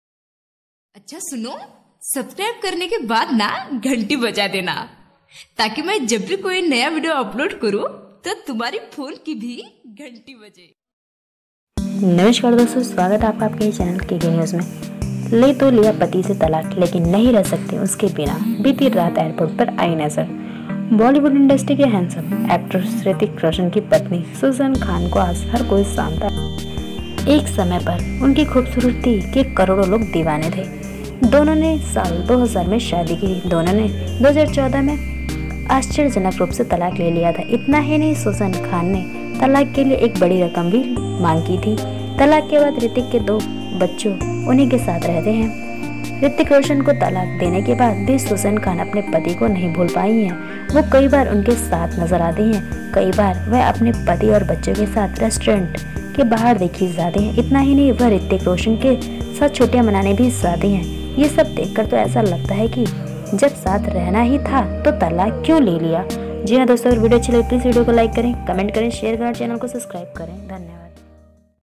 ले तो लिया पति से तलाक लेकिन नहीं रह सकती उसके बिना, बीती रात एयरपोर्ट पर आई नजर
बॉलीवुड इंडस्ट्री के हैंडसम एक्टर ऋतिक रोशन की पत्नी सुजैन खान को आज हर कोई जानता हैं। एक समय पर उनकी खूबसूरती के करोड़ो लोग दीवाने थे

दोनों ने साल 2000 में शादी की। दोनों ने 2014 में आश्चर्यजनक रूप से तलाक ले लिया। इतना ही नहीं, सुजैन खान ने तलाक के लिए एक बड़ी रकम की भी मांग की थी।।तलाक के बाद ऋतिक के दोनों बच्चे उन्ही के साथ रहते हैं।

ऋतिक रोशन को तलाक देने के बाद भी सुजैन खान अपने पति को नहीं भूल पायी हैं, वो कई बार उनके साथ नजर आती हैं। कई बार वह अपने पति और बच्चों के साथ रेस्तरां के बाहर देखी जाती हैं। इतना ही नहीं, वह ऋतिक रोशन के साथ छुट्टियां मनाने भी जाती हैं। यह सब देखकर ऐसा लगता हैं की जब साथ रहना ही था तो तलाक लिया क्यू।